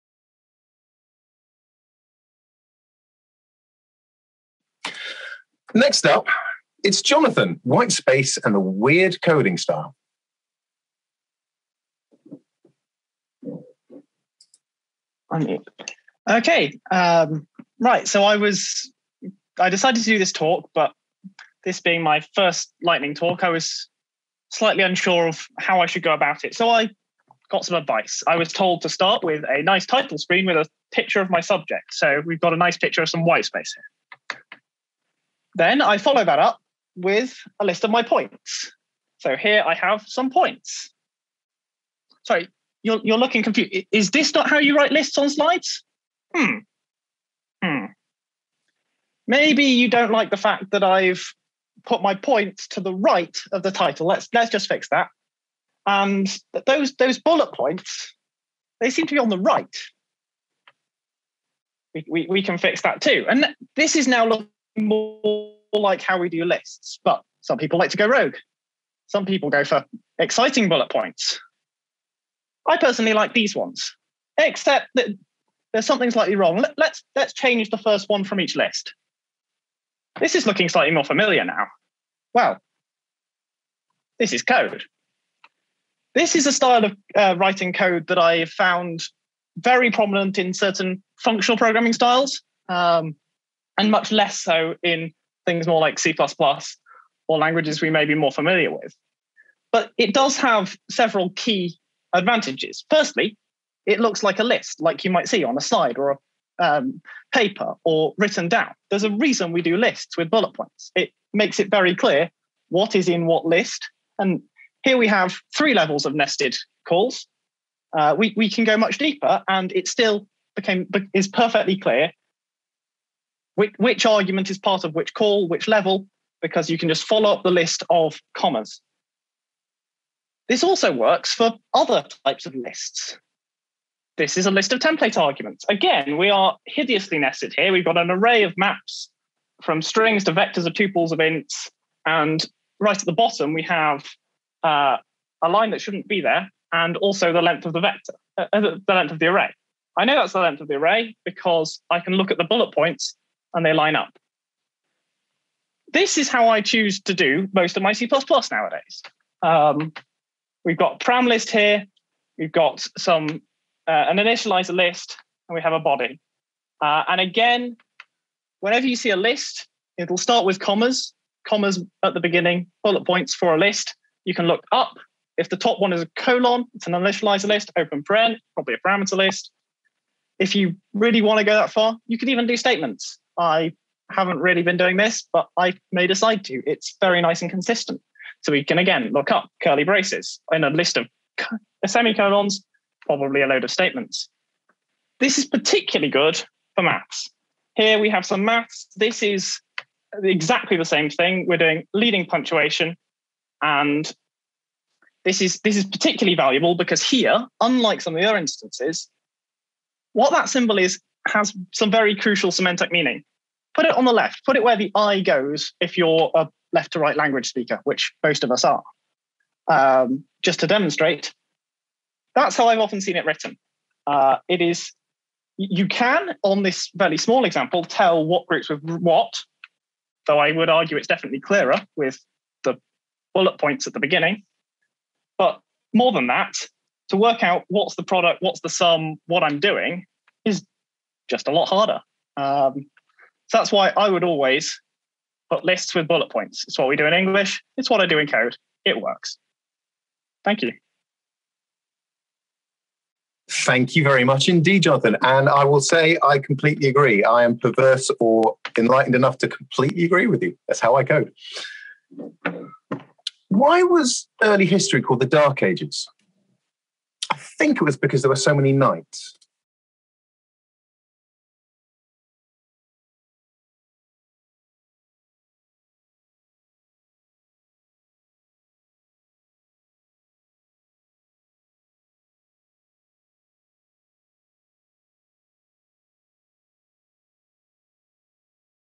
Lightning Talk: Whitespace and the Weird Coding Style - Jonathan Tanner [ ACCU 2021 ]
Whitespace and the Weird Coding Style

Jonathan Tanner

Future Conferences:
ACCU 2022 Spring Conference, Bristol (UK), Marriott City Centre:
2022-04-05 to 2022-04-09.